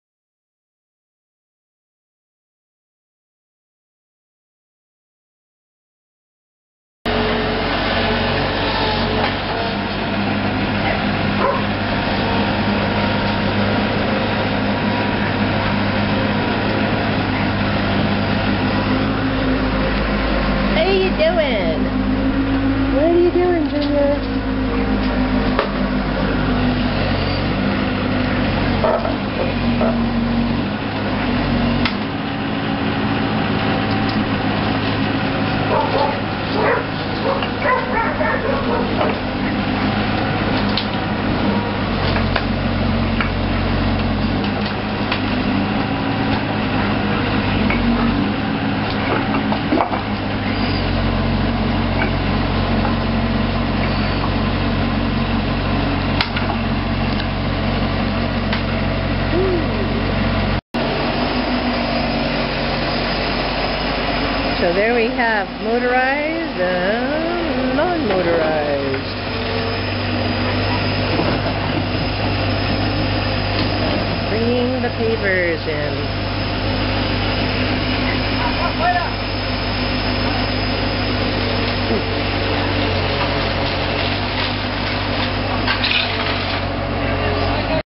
Day3_Driveway.wmv
Day 3: Driveway base layers finished. Pavers arrived and some were installed.